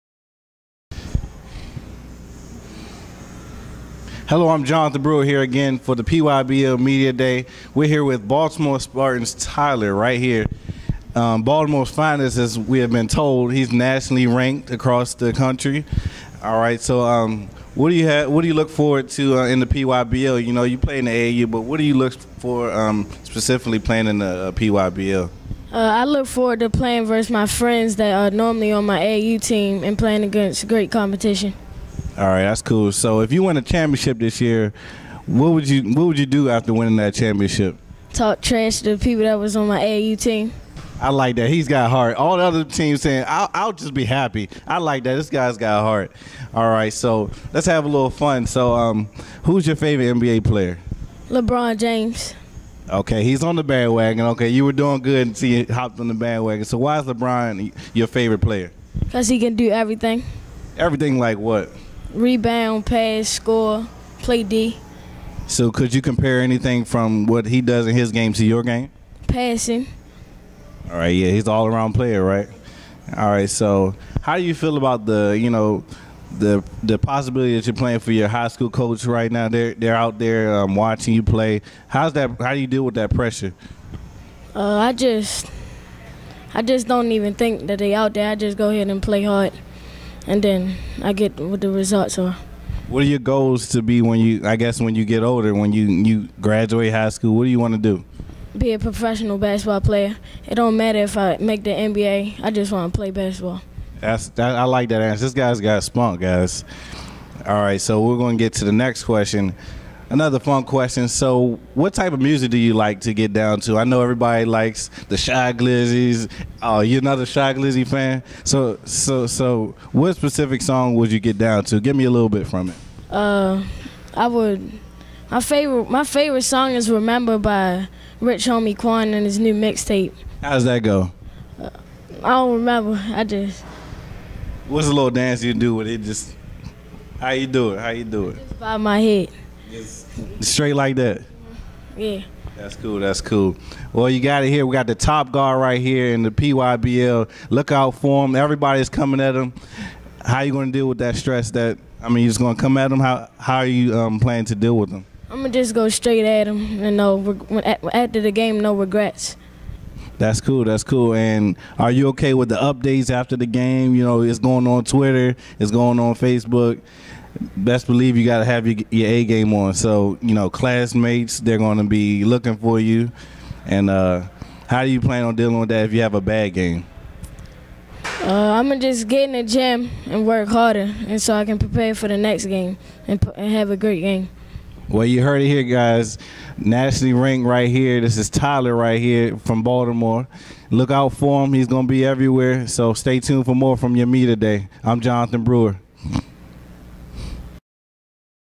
PYBL Tyler Brelsford Interview
Baltimore Spartans Tyler Brelsford  (Class of 2019) pre-season PYBL Summer League interview on Sunday, June 1, 2014 interviewed by Jonathan Brewer